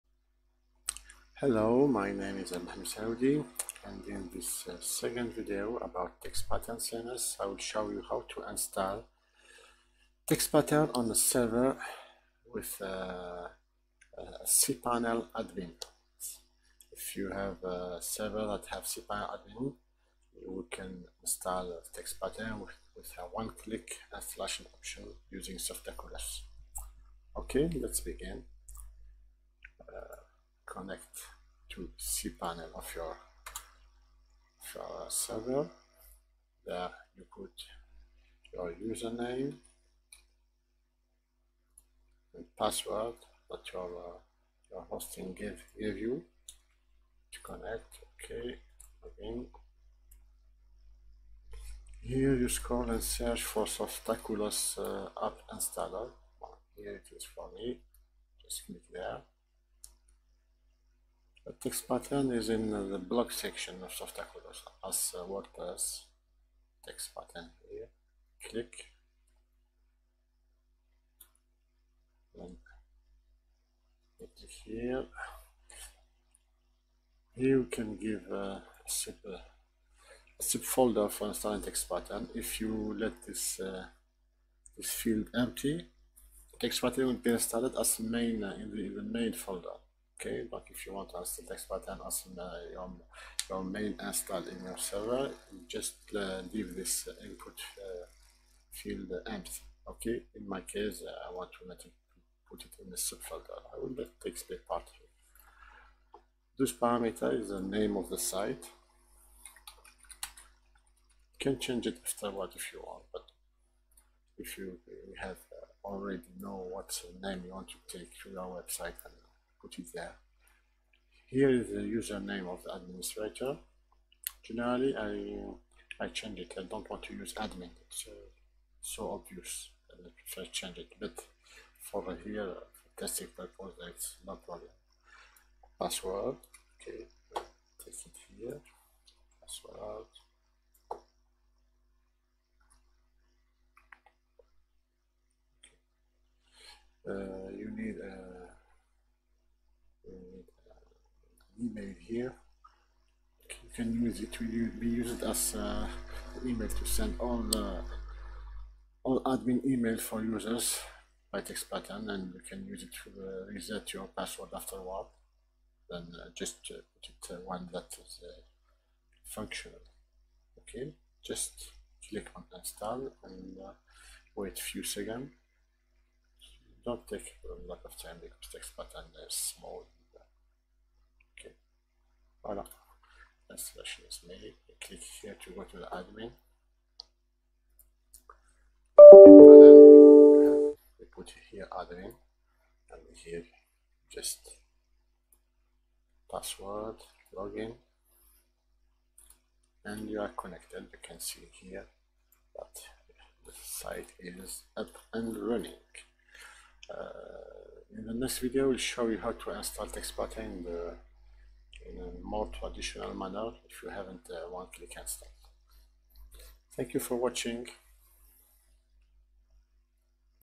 Textpattern CMS Tutorial 2 OneClick install
Installation of Textpattern CMS on a server with a cpanel admin server using the one click Softaculos App Installer.